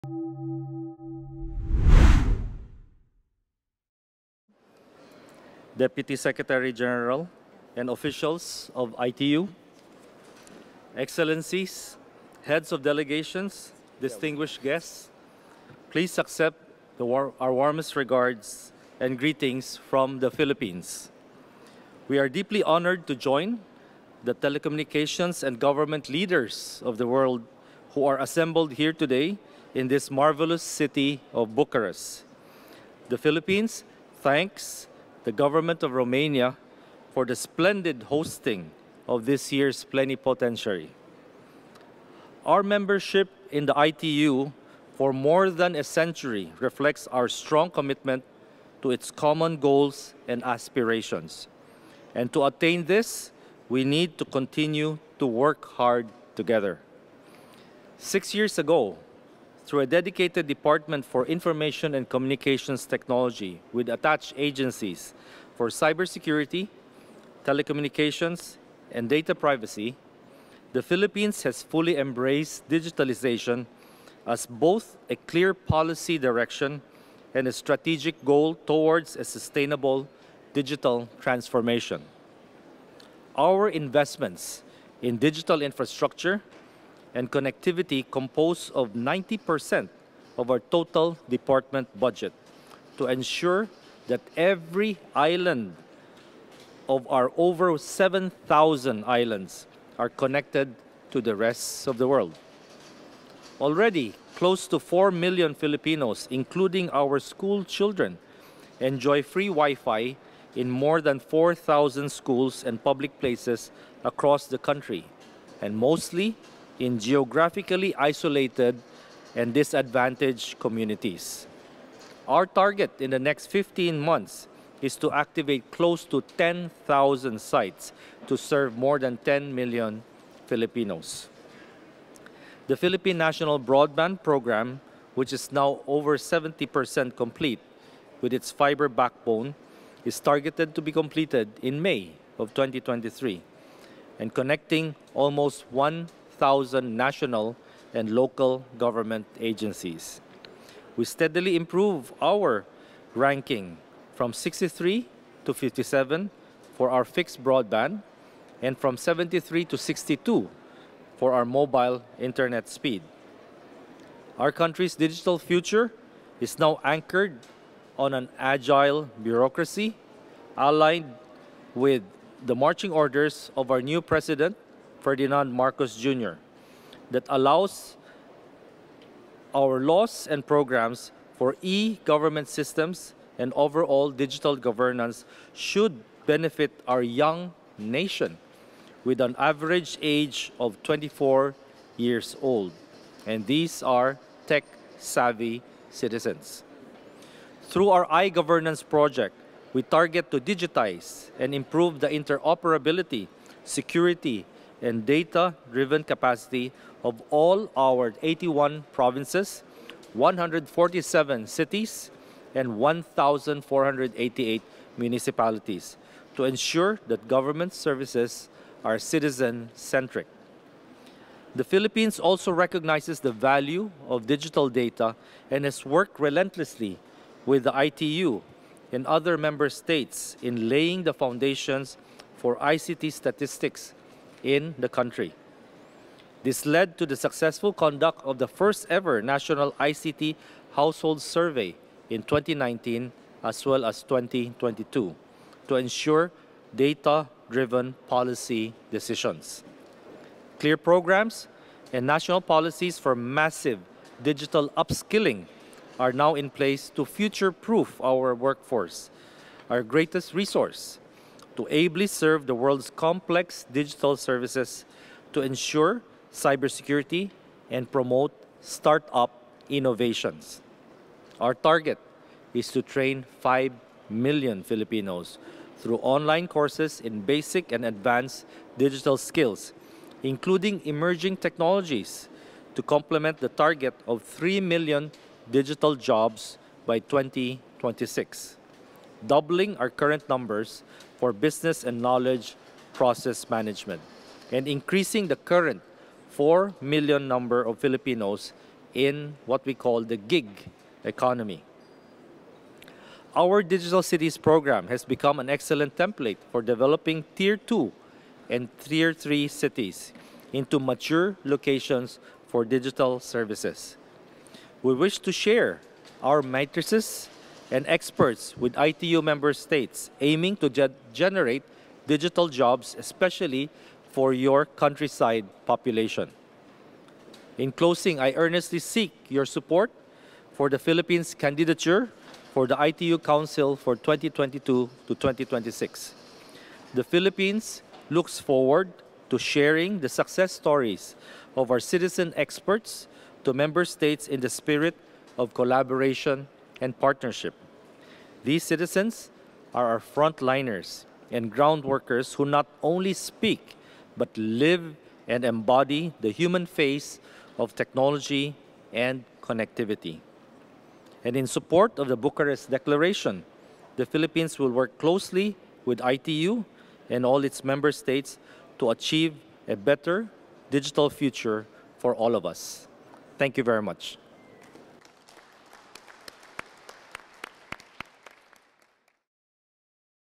ITU PP-22 Policy statement: H.E. Mr Ivan John Uy, Philippines
Policy statement delivered by H.E. Mr Ivan John Uy, Secretary of the Department of Information and Communications of Philippines at the ITU Plenipotentiary Conference 2022 (PP-22), Bucharest.